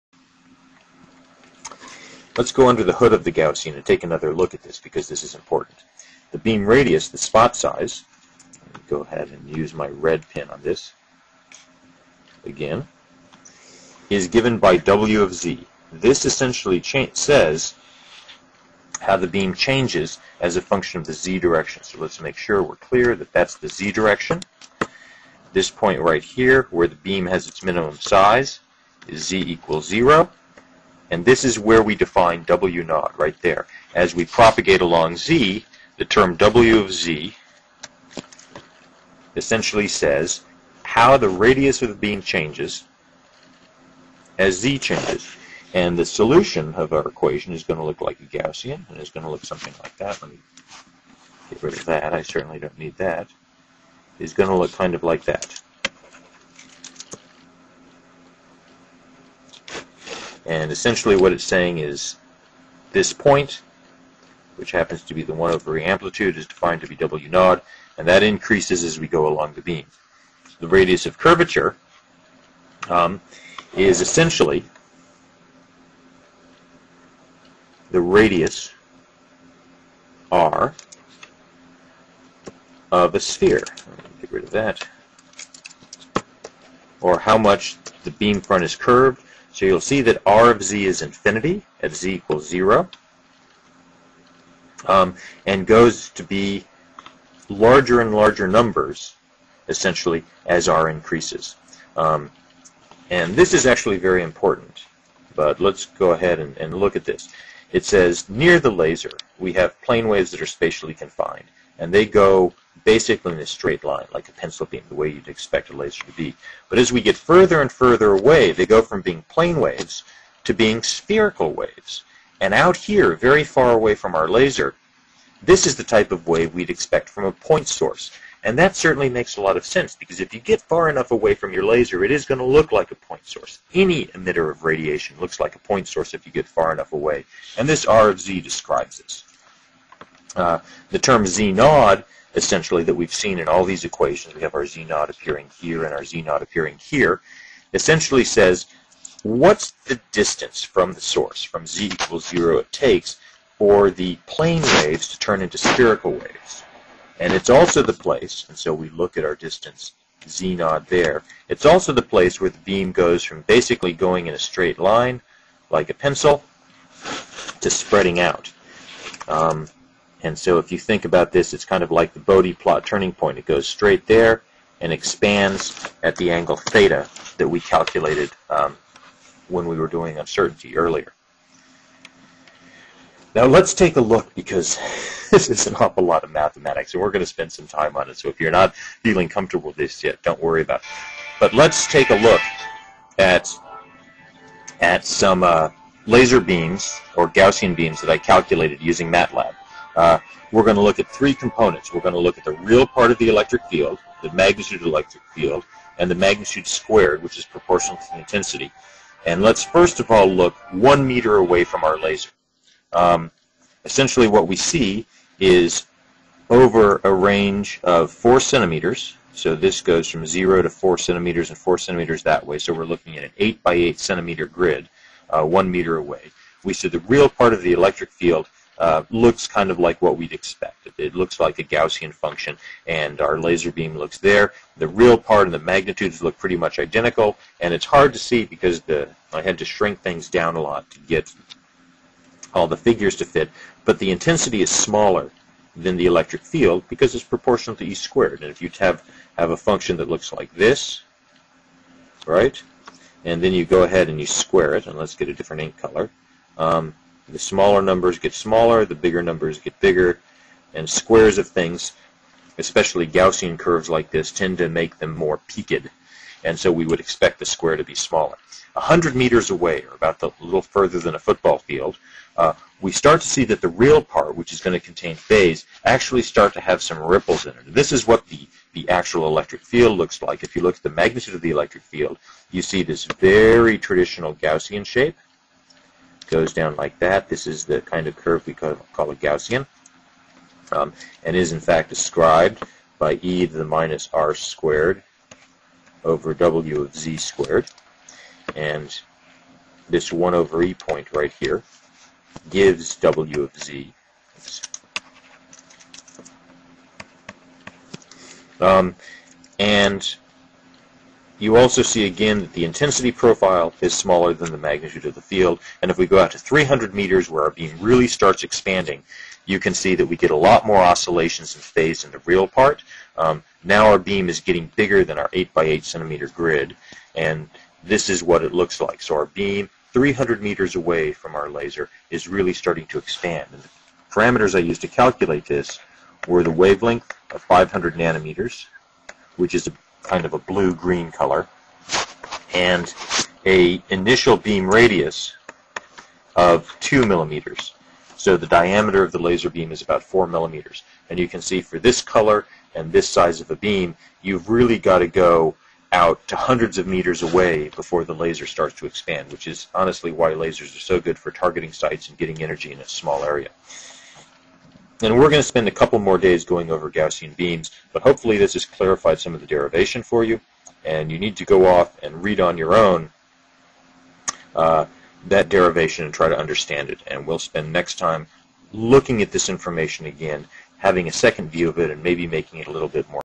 Gaussian Beams- A review, Part III
This video reviews the first part of chapter 3 of Verdeyen's "Laser Electronics" on the theory of Gaussian beams.  Those who don't have the text to follow along might want to skip to part III.  The video is design to support a course in lasers in an undergraduate electrical engineering program.